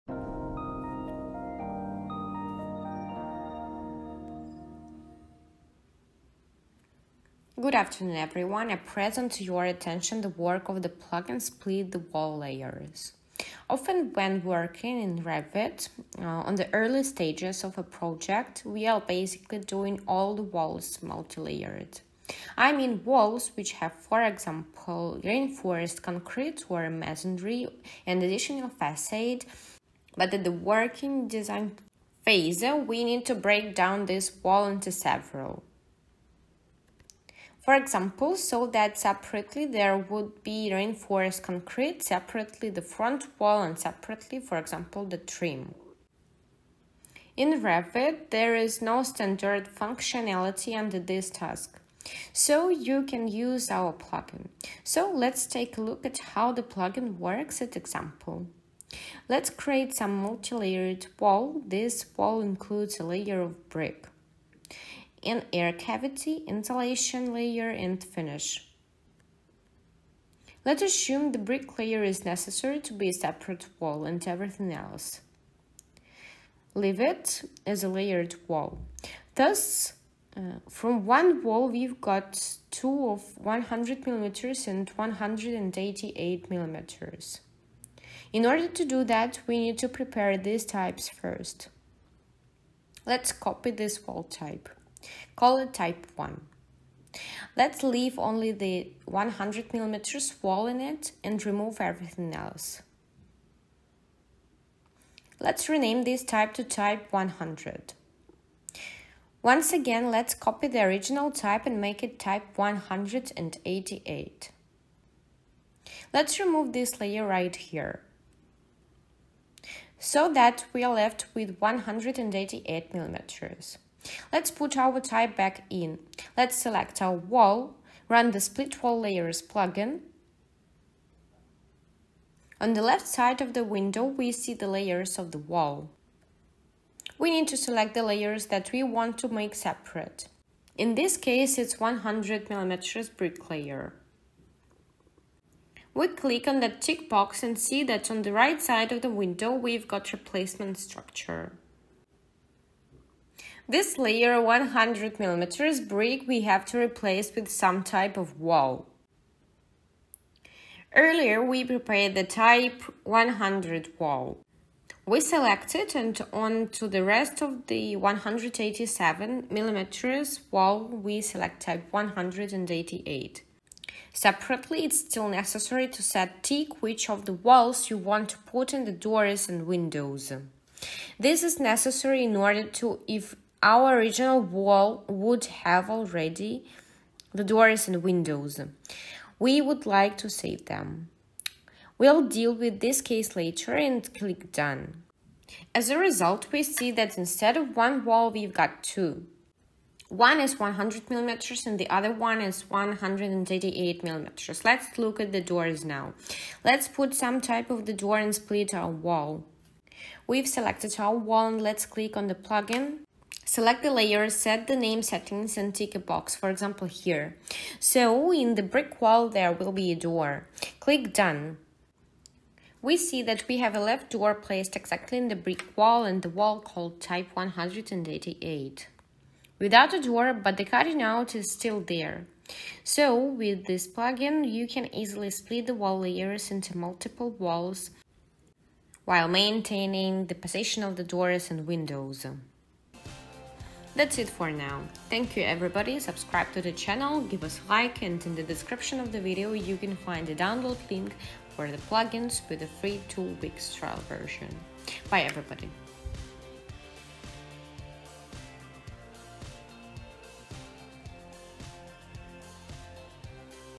Revit plugin Split multilayer walls
The plugin allows to divide multi-layered architectural walls into multiple single or multi-layered walls, depending on how you configure it